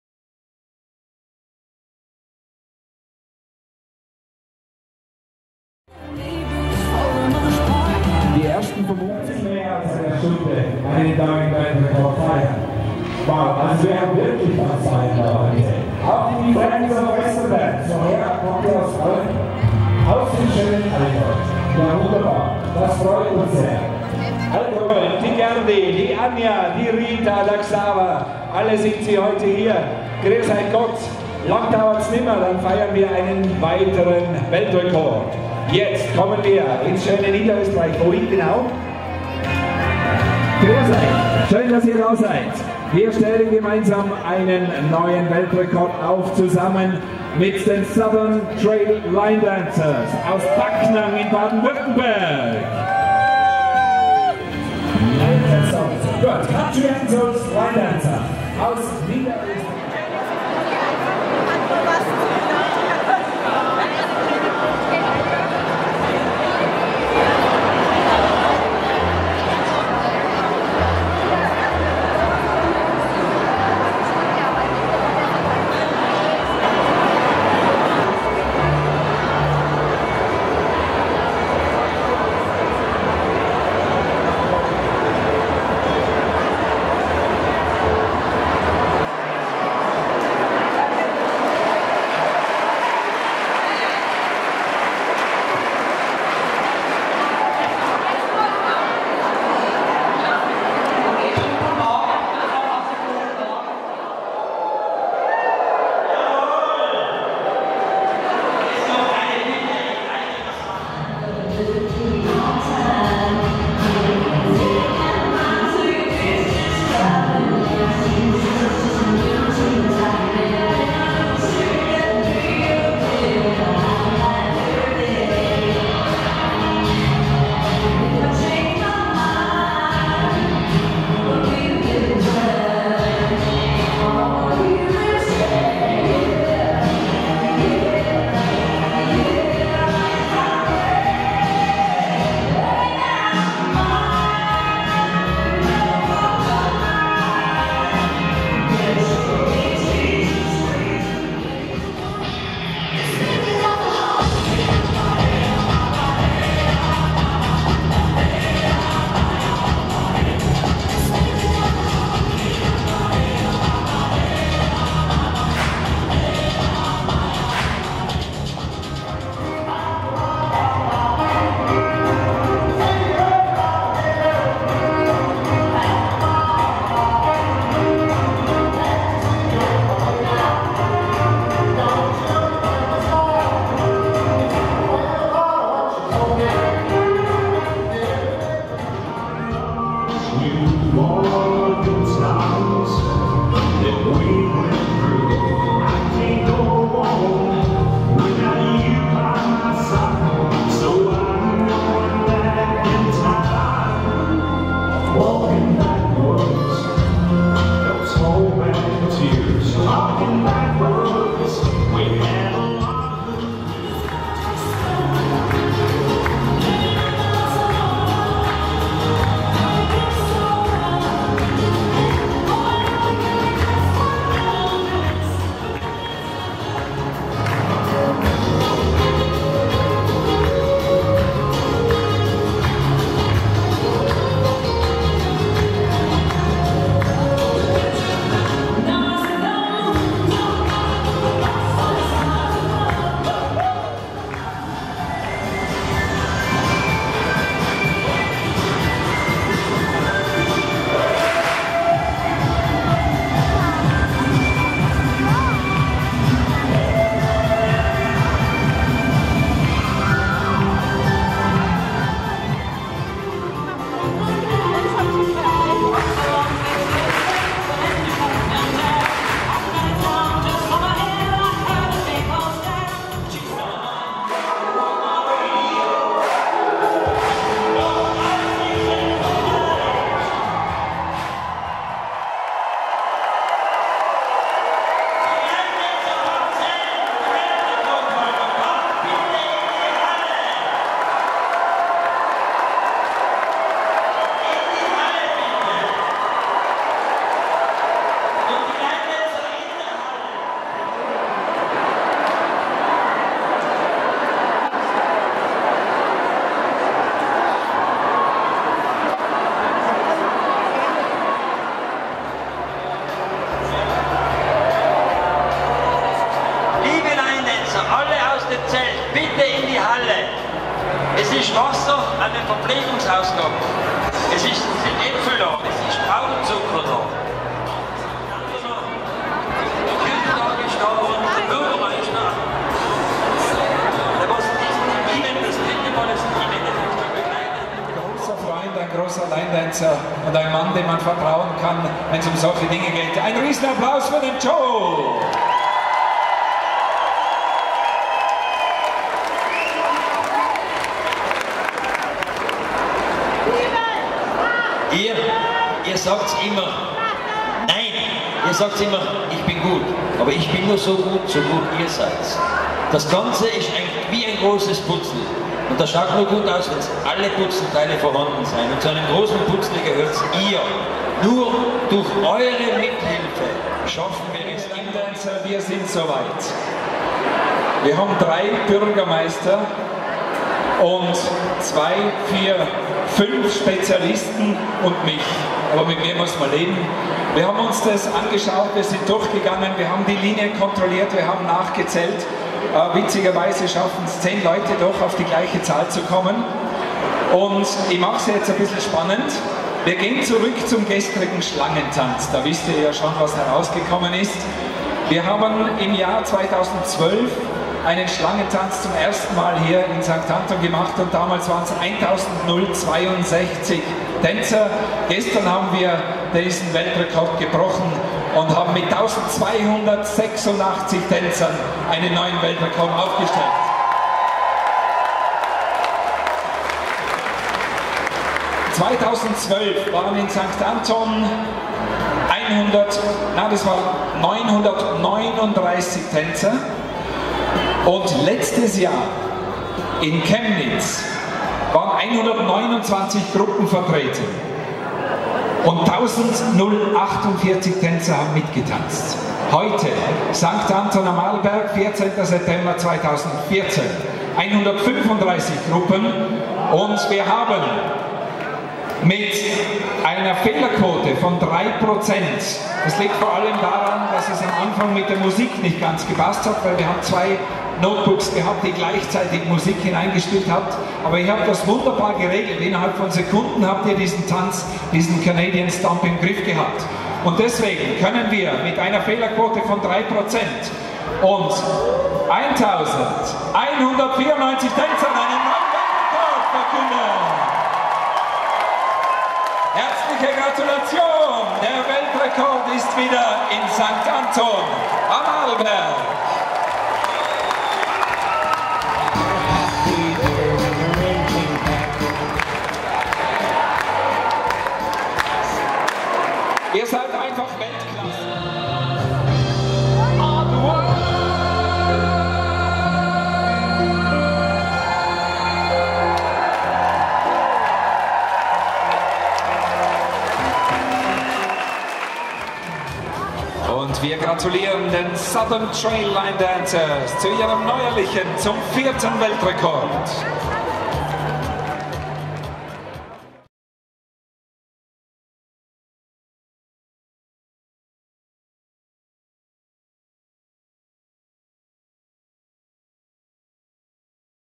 Linedance Weltrekord 2014
Die Southern Trail Linedancers beim Weltrekordversuch 2014 in St. Anton